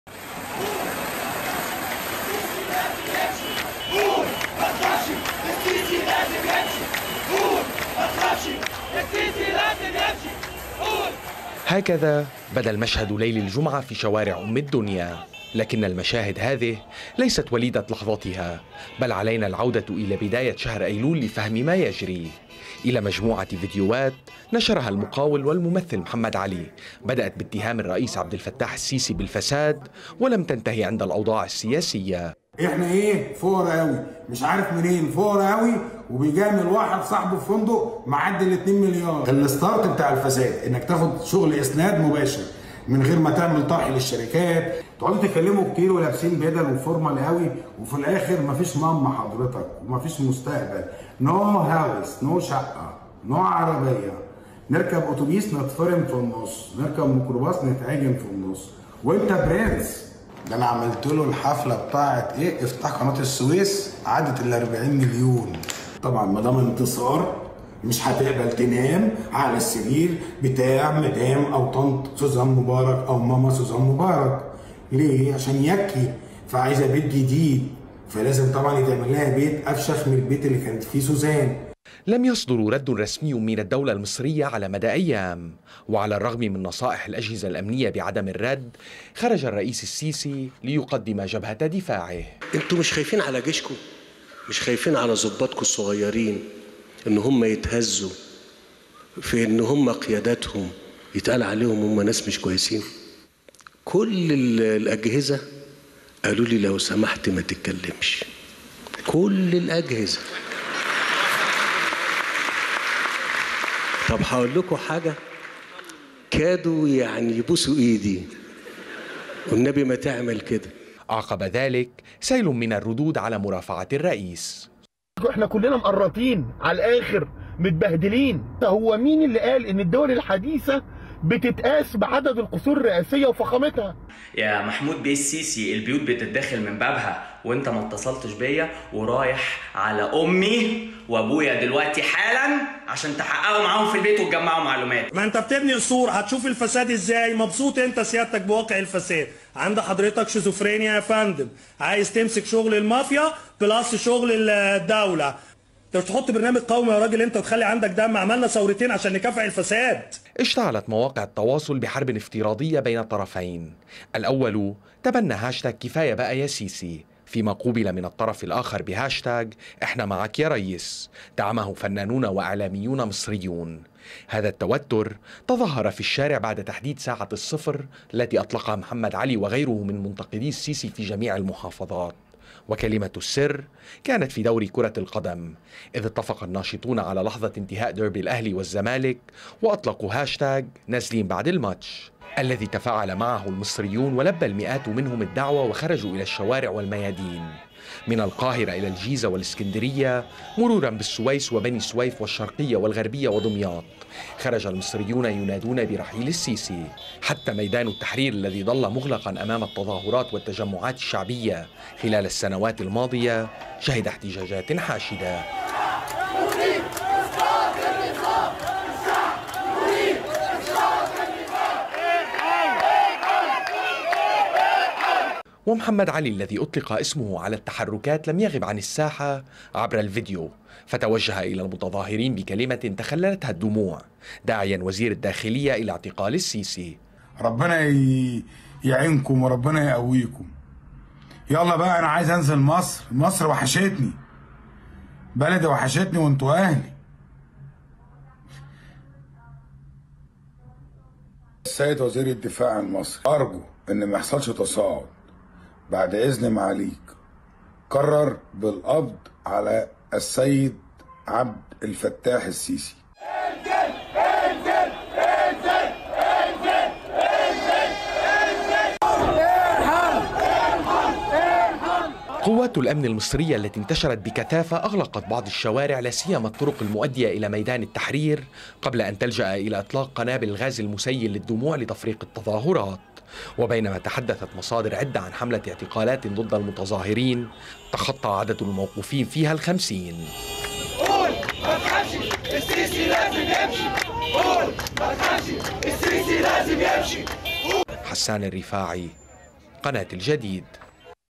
ثورة محمد علي أطلقت بصفارة كرة قدم.. فإلى أين يصل "الماتش" مع السيسي؟ - حسان الرفاعي
خرج المصريون الى الشوارع ودموع محمد علي تواكبهم من الخارج، والشعار في مختلف المدن واحد: رحيل السيسي.
بدأت القصة في شهر أيلول عندما نشر المقاول والممثل محمد علي مجموعة فيديوهات بدأت باتهام الرئيس عبد الفتاح السيسي بالفساد. لم يصدر رد رسمي من الدولة المصرية على مدى أيام، ورغم نصائح الأجهزة الأمنية بعدم الرد، خرج السيسي ليقدم جبهة دفاعه.
اشتعلت مواقع التواصل بحرب افتراضية، ترجمت في الشارع بعد تحديد ساعة الصفر التي أطلقها محمد علي وغيره من منتقدي السيسي في جميع المحافظات وكلمة السر كانت في دوري كرة القدم.